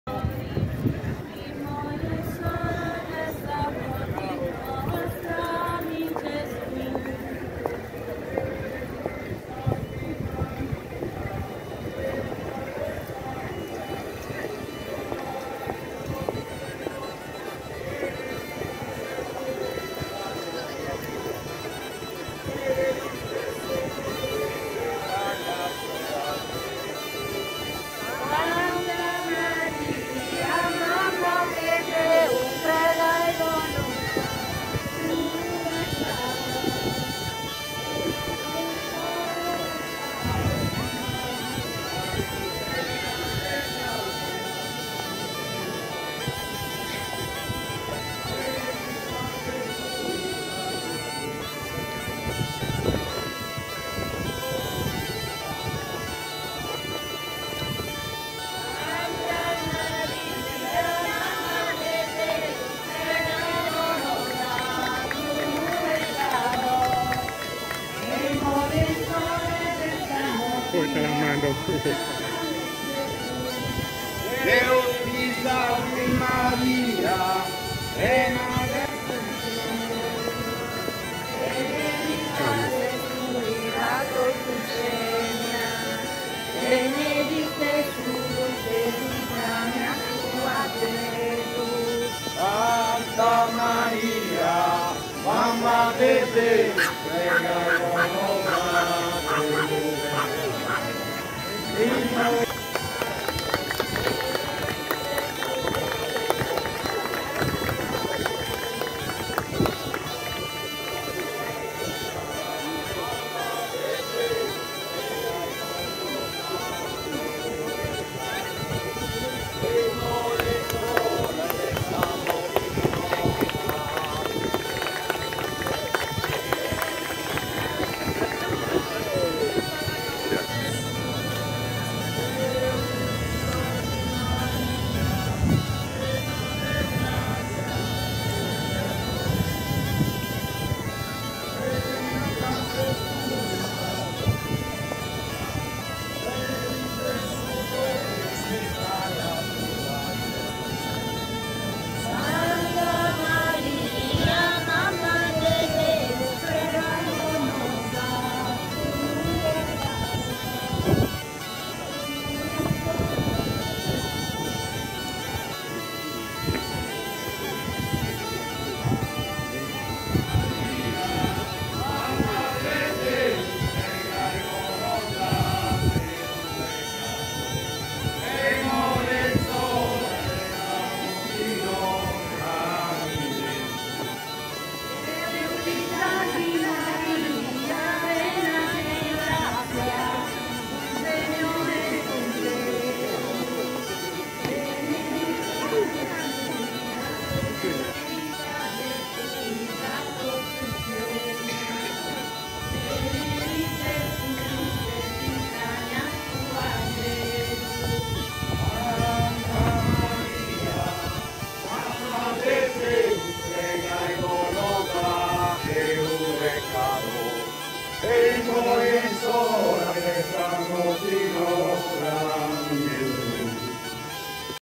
La Pro Loco Marese Maracalagonis alla 664ª Edizione della Festa di Sant'Antioco Martire 2023
La Pro Loco Marese Maracalagonis alla 664ª edizione della Festa di Sant'Antioco Martire, Patrono della Sardegna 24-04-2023.